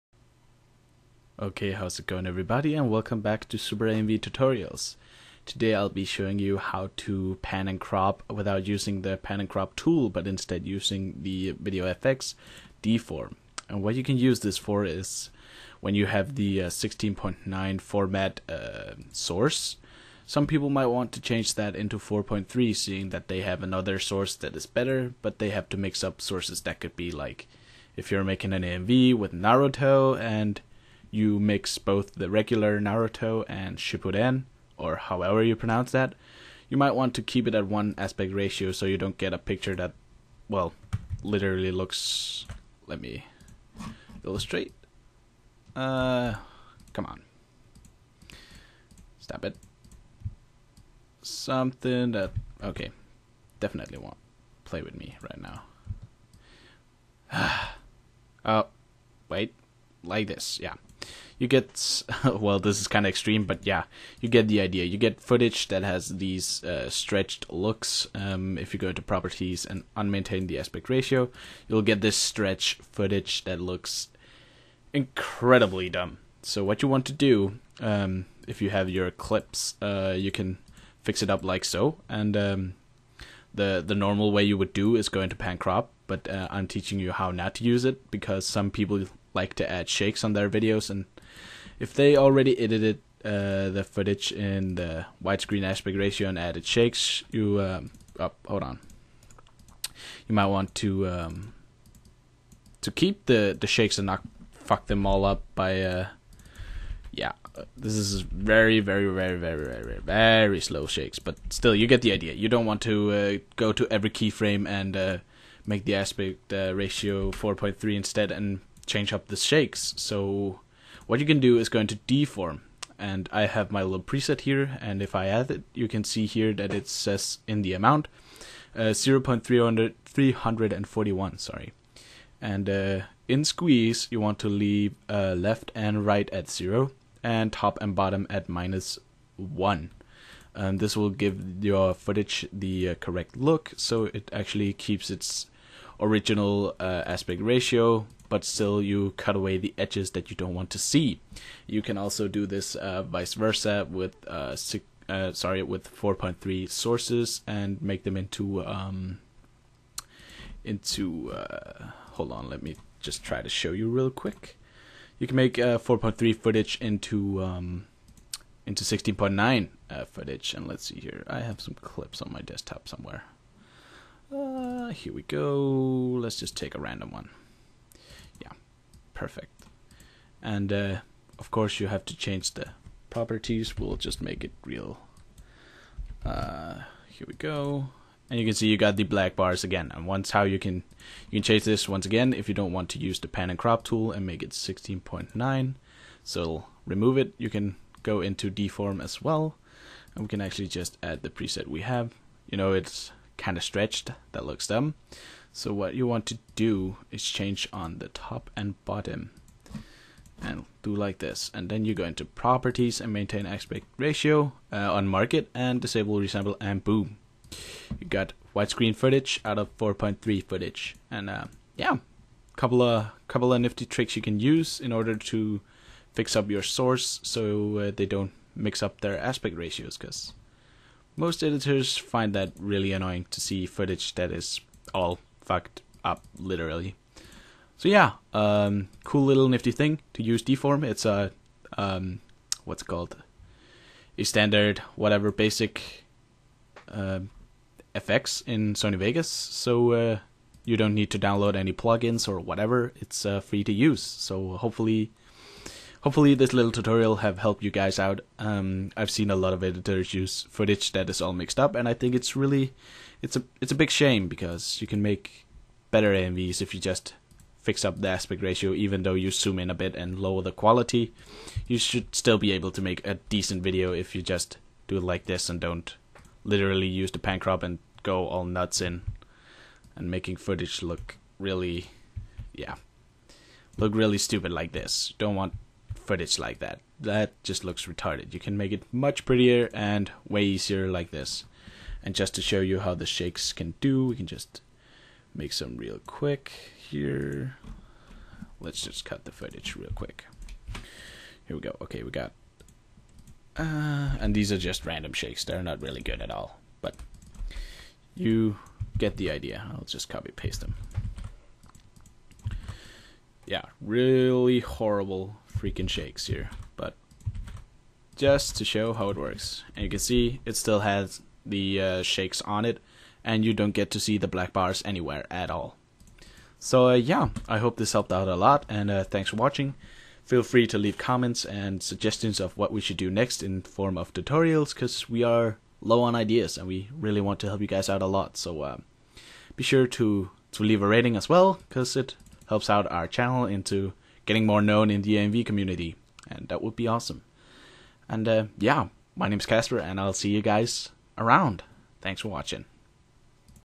Deform - How To Change Aspect Ratio (Without Using Pan/Crop)
Tutorial on how to make change the footage youve got from 4:3 to 16:9 and vise versa without using the pan/crop tool
Other Guys